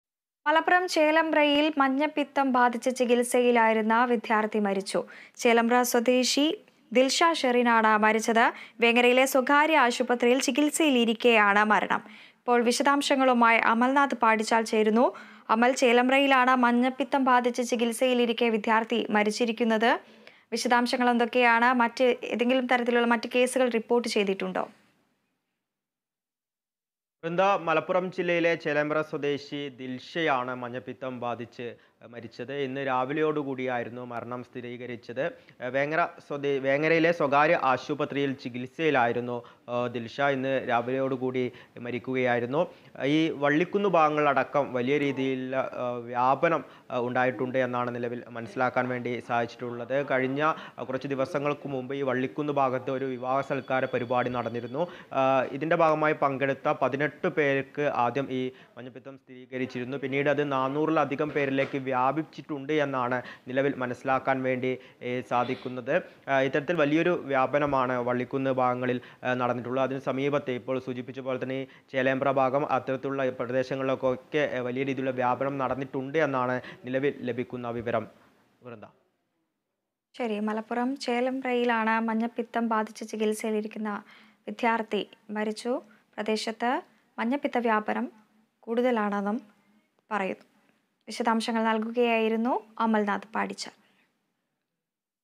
മലപ്പുറം ചേലേമ്പ്രയില്‍ മഞ്ഞപ്പിത്തം ബാധിച്ച് ചികിത്സയിലായിരുന്ന വിദ്യാര്‍ത്ഥി മരിച്ചു
ചേലേമ്പ്രയില്‍ മഞ്ഞപ്പിത്തം ബാധിച്ചു ചികിത്സയിലായിരുന്ന വിദ്യാര്‍ത്ഥി മരിച്ചു. ചേലേമ്പ്ര സ്വദേശി ദില്‍ഷ ഷെറിന്‍ ആണ് മരിച്ചത്. വേങ്ങരയിലെ സ്വകാര്യ ആശുപത്രിയില്‍ ചികിത്സയിലിരിക്കെയാണ് മരണം.

About COA